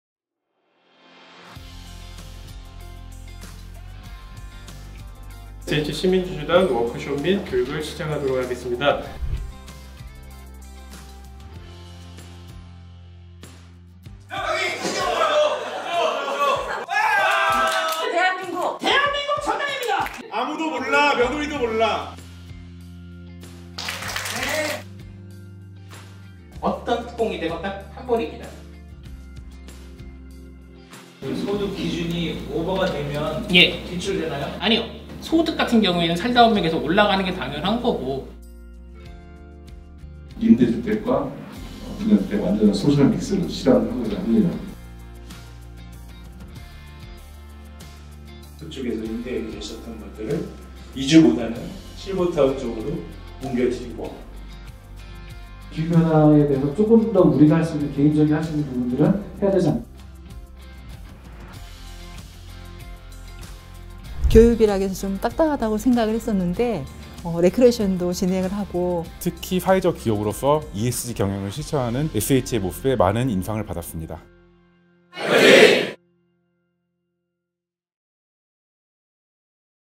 2023 SH 시민주주단 워크숍 및 교육 현장 스케치!
항상 서울시민과 함께하는 서울주택도시공사!
2023 SH 시민주주단 워크숍 및 교육이
5월 30일 개최되었습니다.
SH 시민주주단의 환한 웃음이 가득한 현장을 소개합니다!